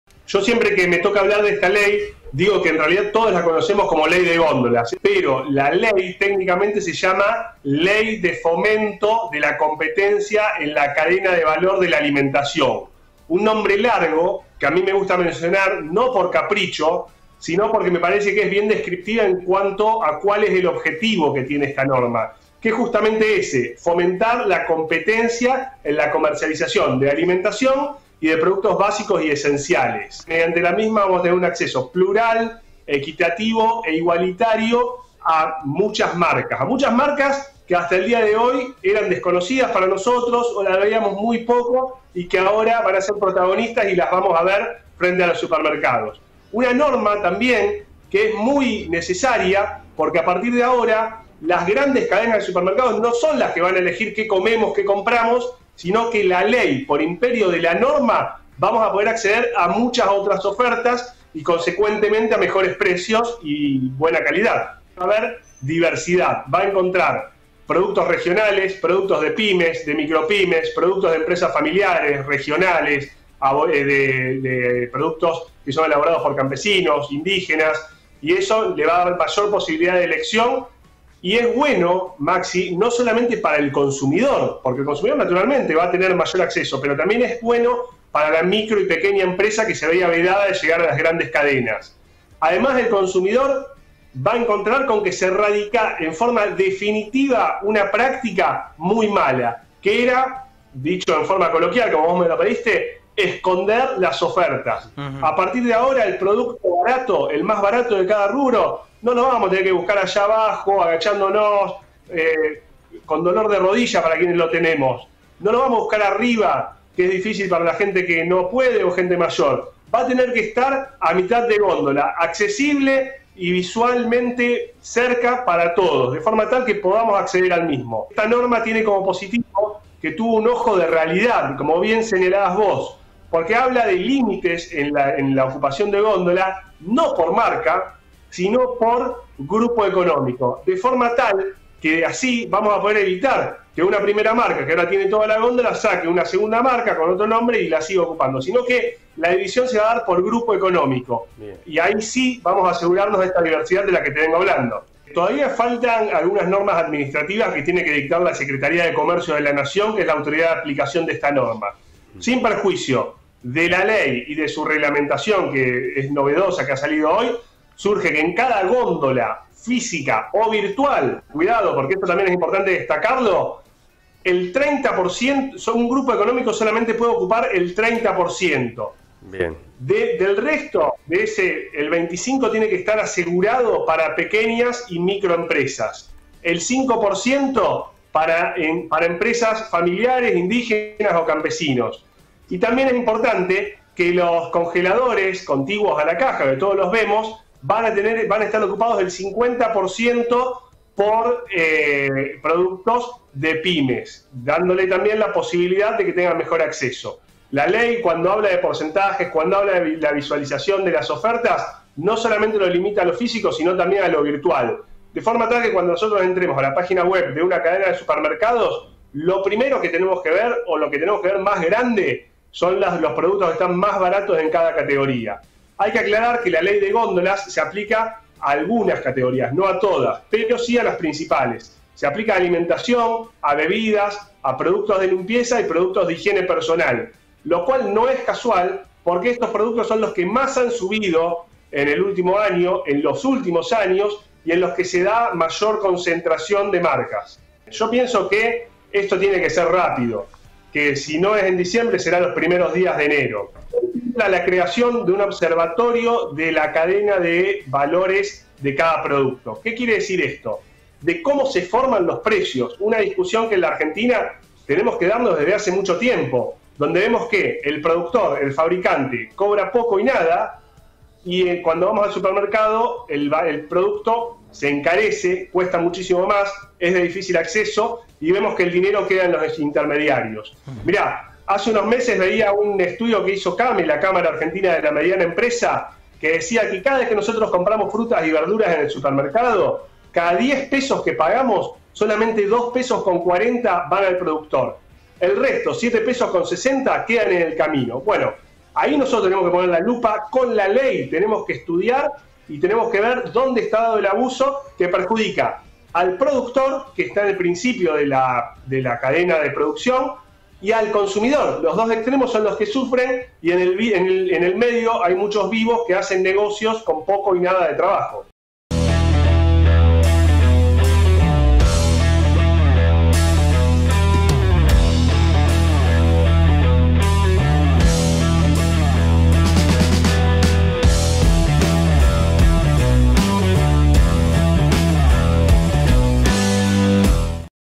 "Ley de góndolas": de qué se trata y cómo nos cambiará la forma de comprar a los consumidores
Ignacio Pandullo, Coordinador Jurídico del CESyAC, charló en el mediodía de Sensibilizados con nosotros y respondió todas nuestras dudas.